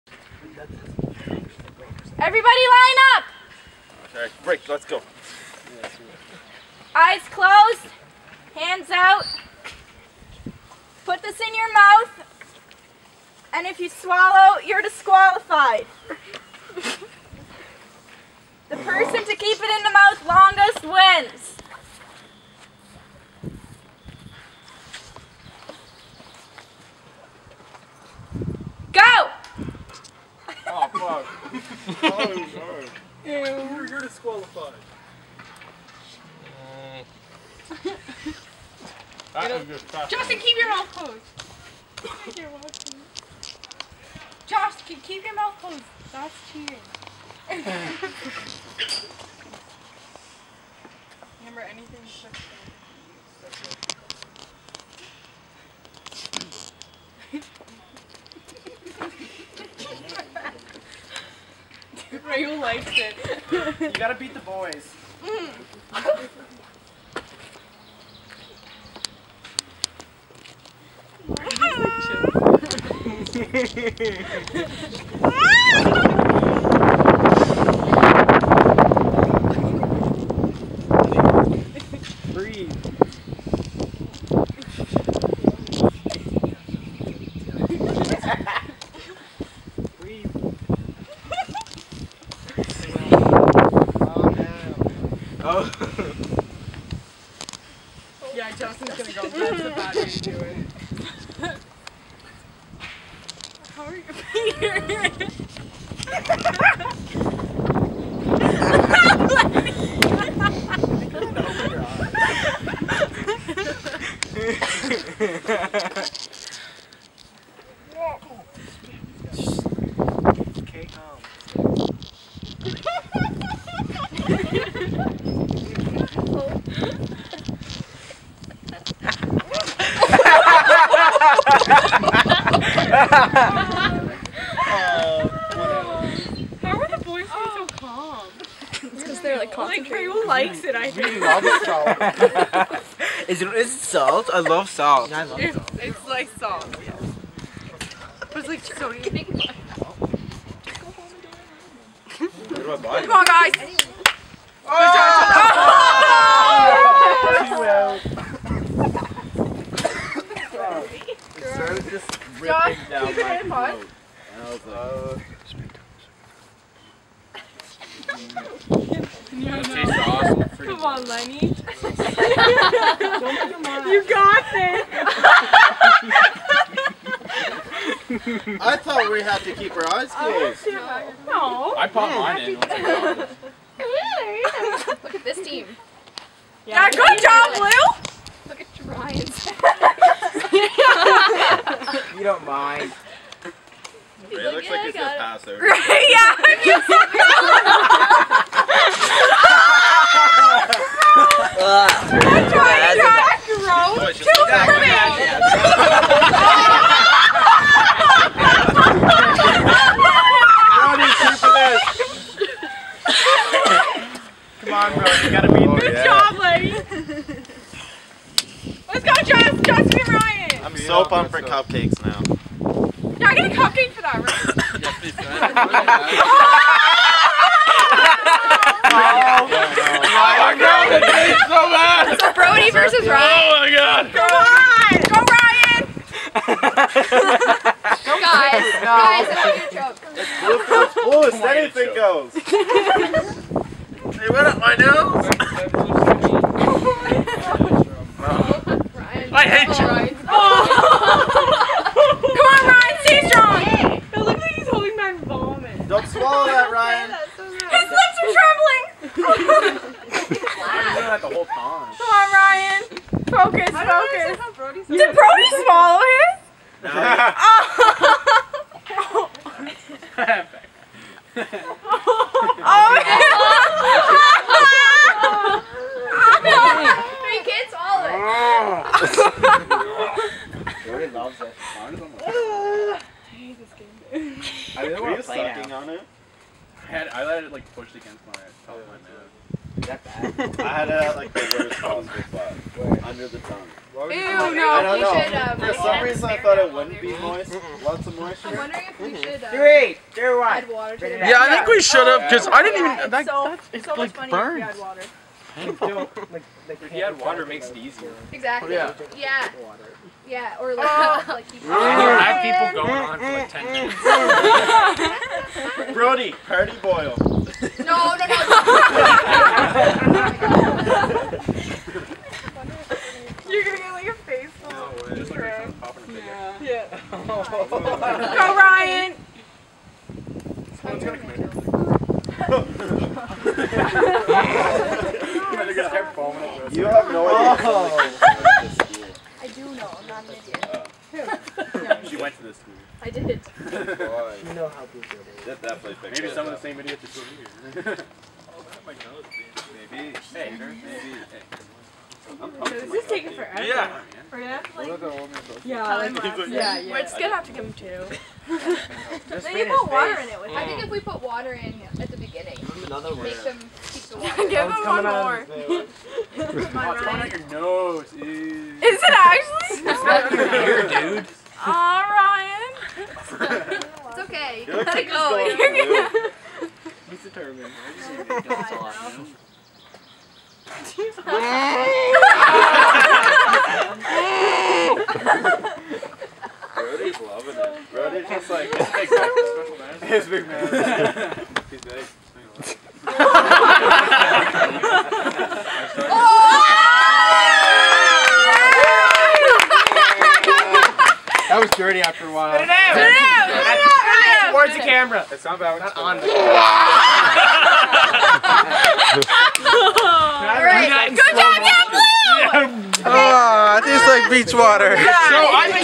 The Challenge
Back in Bluefield Memories, when the students of a class have to create a video to advertise their event coming up.  This was never published.